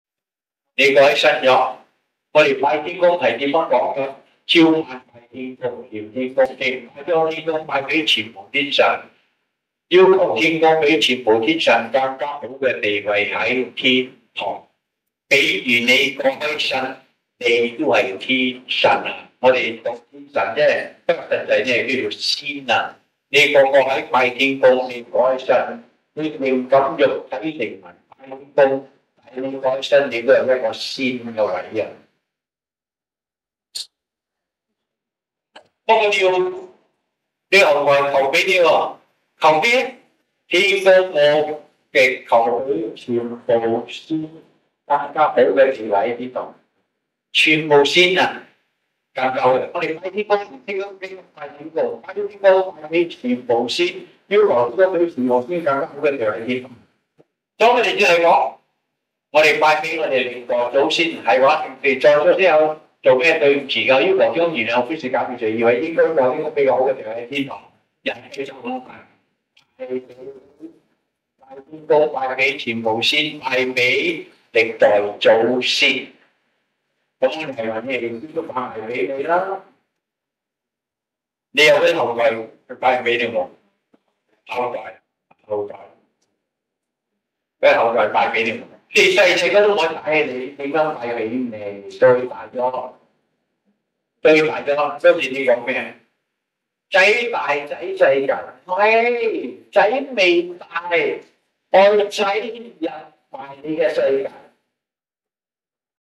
29AB 2000. (C) Praying for our ancestors.
1. Xian Sheng shared with the followers our common request in our prayers for the deceased.
2. Xian Sheng explained that we pray twice a day, typically upon waking in the morning and before retiring at night. After reciting the Baitiangong prayer, we request the CREATOR to bless all the saints with a higher plane of Heaven.
3. When Baitiangong followers, who pray to the CREATOR with their mind, body and soul pass away, they attain a place in Heaven as saints.
4. However, Xian Sheng emphasised that we require our descendants to pray for us. They would also pray following the common request - we request the CREATOR to bless all the saints with a higher plane of Heaven. Xian Sheng stressed that we pray for all the saints in Heaven.
5. After we pray for all the saints, we pray for our ancestors. We pray to the CREATOR to forgive, pardon and lessen the sins of our ancestors. If deserving, to give them a good place in Heaven. We continue to pray as follows for the rest of our lives for all the saints and our ancestors.
6. For individuals who are already saints in Heaven, our prayers encompass them as well.
7. Xian Sheng inquired if we have any descendants or children to pray for us. He raised the question that if we do not involve our children in Baitiangong activities when they are young, can we expect them to pray for us when they have grown up?
8. If our response contends that our children have grown up and live in their own world, thus not following their parents in praying to the CREATOR, Xian Sheng rebuts by stating that we are mistaken. He asserts that we should have ensured, when our children were still young, to include them in our world.